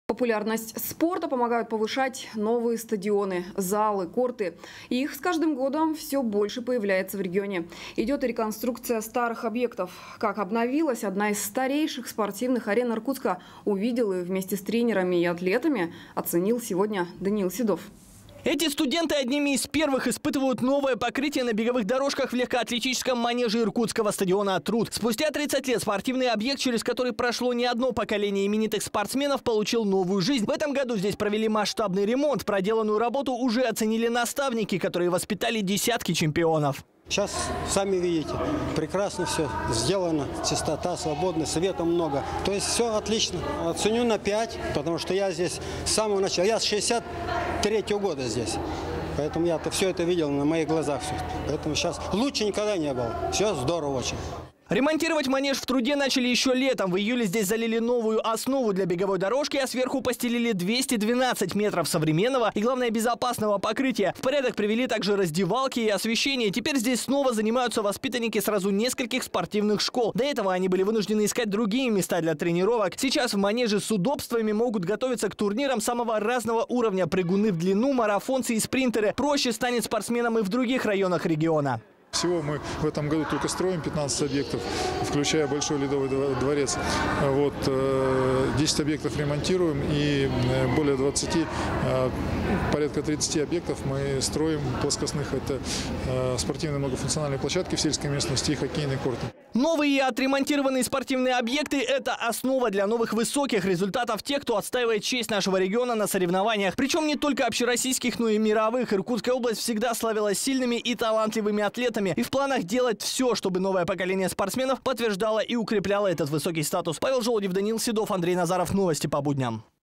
Масштабный ремонт завершён в легкоатлетическом манеже стадиона "Труд" в Иркутске
Популярность спорта помогают повышать новые стадионы, залы, корты. Их с каждым годом всё больше появляется в регионе. Идёт и реконструкция старых объектов. Как обновилась одна из старейших спортивных арен Иркутска, увидел и вместе с тренерами и атлетами оценил сегодня Даниил Седов.

Эти студенты одними из первых испытывают новое покрытие на беговых дорожках в легкоатлетическом манеже иркутского стадиона "Труд". Спустя 30 лет спортивный объект, через который прошло не одно поколение именитых спортсменов, получил новую жизнь. В этом году здесь провели масштабный ремонт. Проделанную работу уже оценили наставники, которые воспитали десятки чемпионов.

ИГОРЬ БРАЖНИК, тренер по лёгкой атлетике: «Сейчас сами видите: всё прекрасно, всё сделано. Чистота, свобода, света много. Всё отлично! Оценю на пять, потому что я здесь с самого начала. Я с 63-ого года здесь. И поэтому всё на моих глазах было. Лучше никогда не было! Всё здорово очень!»

Ремонтировать манеж в "Труде" начали ещё летом. В июле здесь залили новую основу для беговой дорожки, а сверху постелили 212 метров современного и, что важно, безопасного покрытия. В порядок привели также раздевалки и освещение. Теперь здесь снова занимаются воспитанники сразу нескольких спортивных школ. До этого они были вынуждены искать другие места для тренировок. Сейчас в манеже с удобствами могут готовится к турнирам самого разного уровня прыгуны в длину, марафонцы и спринтеры. Проще станет спортсменам и в других районах региона.

ИЛЬЯ РЕЗНИК, министр спорта Иркутской области: «Всего мы в этом году только строим 15 объектов. Это включая большой ледовый дворец. 10 объектов ремонтируем и более 20, даже порядка 30 объектов мы строим плоскостных. Это спортивные многофункциональные площадки в сельской местности и хоккейные корты».

Новые и отремонтированных спортивные объекты - это основа для новых высоких результатов тех, кто отстаивает честь нашего региона на соревнованиях. Причём не только общероссийских, но и мировых. Иркутская область всегда славилась сильными и талантливыми атлетами. И в планах делать всё, чтобы новое поколение спортсменов подтверждало и укрепляло этот высокий статус.